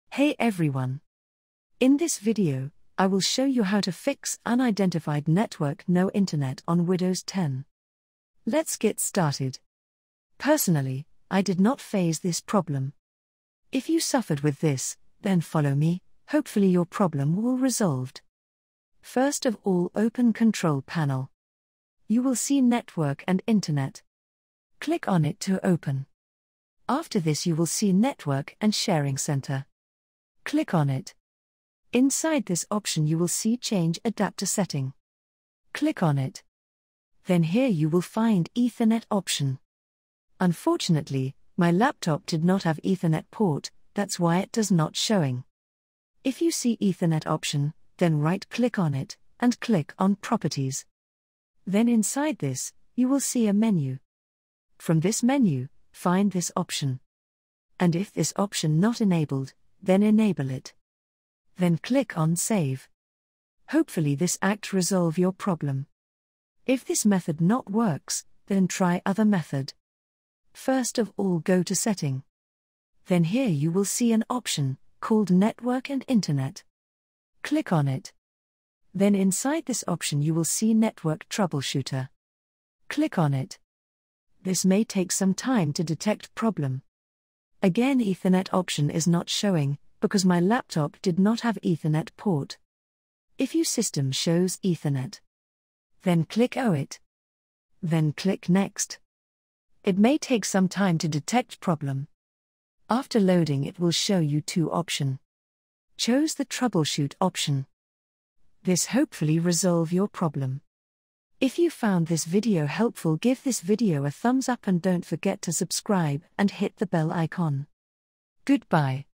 How To Fix Unidentified Network No Internet Issue In Windows 10
Learn how to fix the "Unidentified Network No Internet" issue in Windows 10 with this step-by-step guide. Troubleshoot your network settings and resolve connectivity problems to get back online quickly. Follow these easy methods to diagnose and fix network issues on your Windows 10 PC.